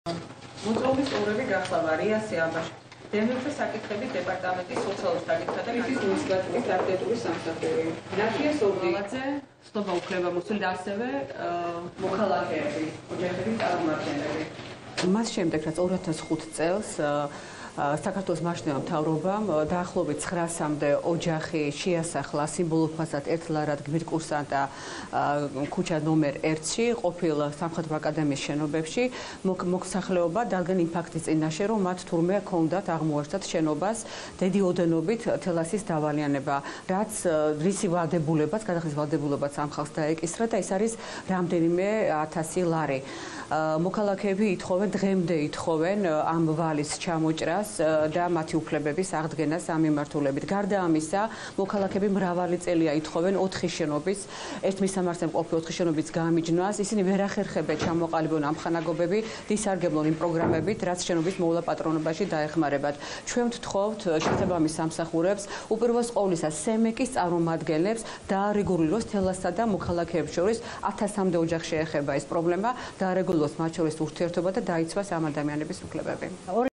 გულიკო ზუმბაძის კომენტარი
თბილისის მუნიციპალიტეტის საკრებულოს ადამიანის უფლებათა დაცვისა და სამოქალაქო ინტეგრაციის კომისიის სხდომაზე  გმირ კურსანტთა ქუჩა N1-ში მცხოვრები მოქალაქეების მდგომარეობის შესახებ და აგრეთვე  „სამოქალაქო თანასწორობისა და ინტეგრაციის სახელმწიფო სტრატეგია და 2016 წლის სამოქმედო გეგმის“ ფარგლებში, მუნიციპალიტეტის სამოქმედო გეგმის შემუშავების საკითხებზე იმსჯელეს.

სხდომაზე, კომისიის წევრებს გმირ კურსანტთა ქუჩა N1-ში მცხოვრები მოსახლეობის წინაშე მდგარი პრობლემატური საკითხების შესახებ ინფორმაცია, კომისიის თავმჯდომარემ გულიკო ზუმბაძემ წარუდგინა.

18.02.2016